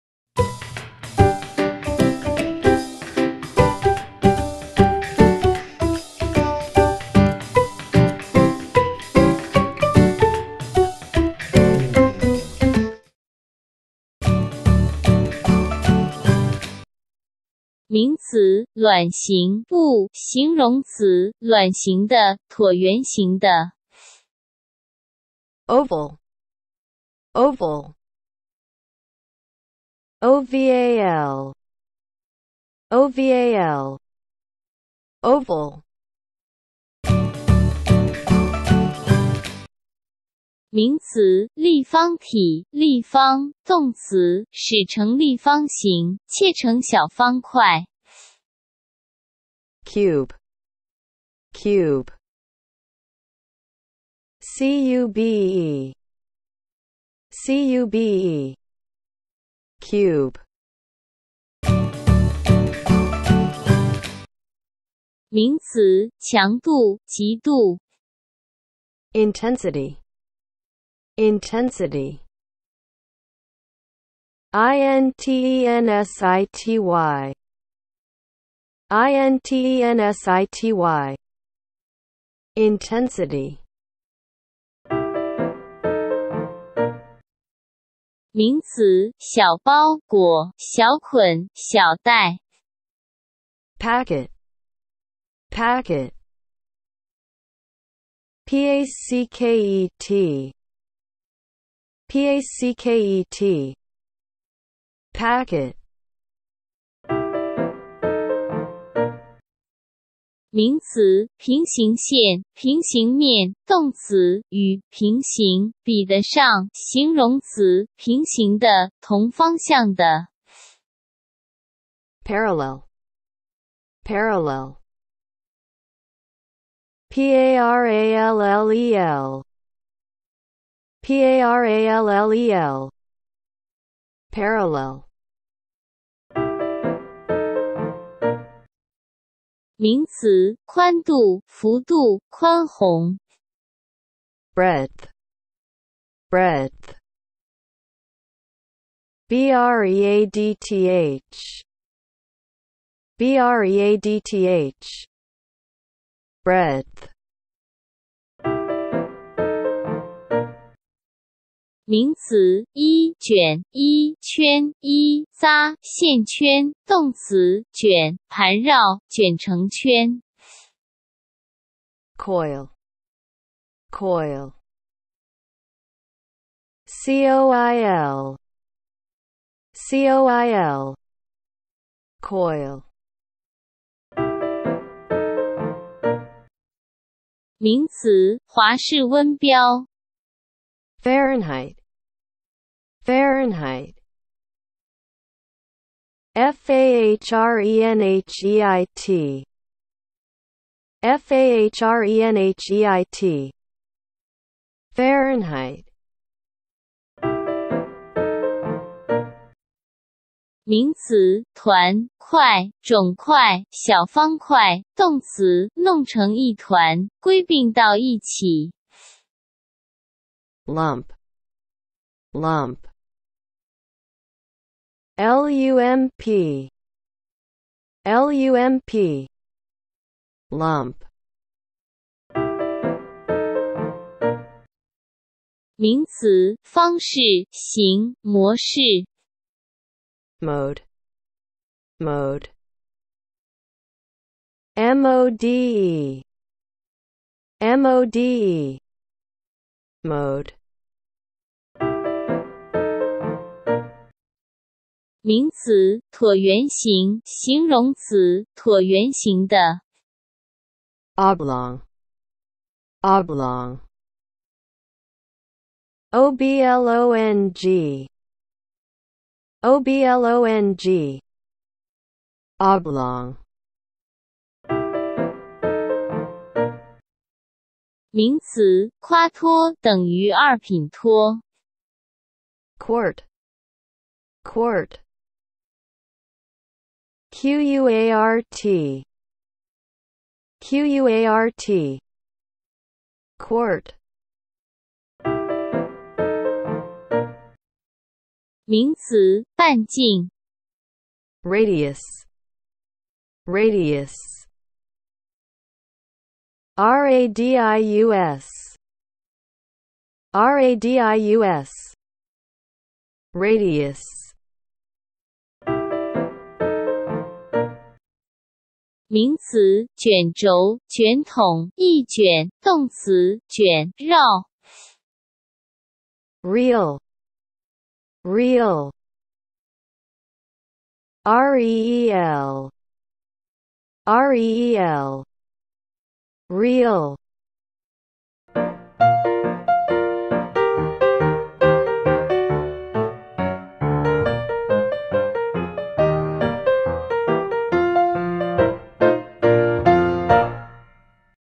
經典單字(高中7000字)-第313單元(中前英後)
【oval】
直接播放：0:14

【cube】
直接播放：0:36

【intensity】
直接播放：1:01

【packet】
直接播放：1:24

【parallel】
直接播放：1:46

【breadth】
直接播放：2:16

【coil】
直接播放：2:38

【Fahrenheit】
直接播放：3:04

【lump】
直接播放：3:27

【mode】
直接播放：3:52

【oblong】
直接播放：4:11

【quart】
直接播放：4:34

【radius】
直接播放：4:55

【reel】
直接播放：5:15